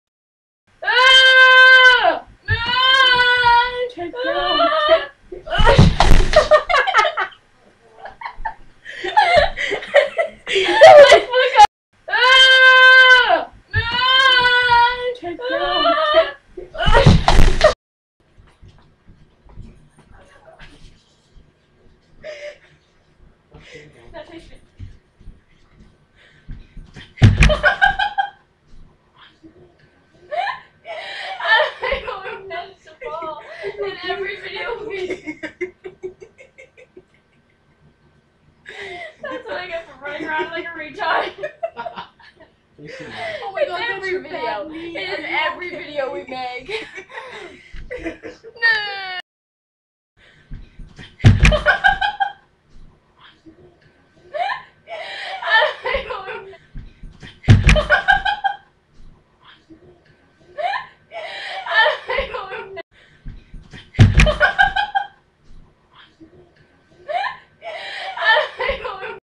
Why am i not ok..
I am constantly falling, in EVERY video we make we always have bloopers these are 2 of our recent ones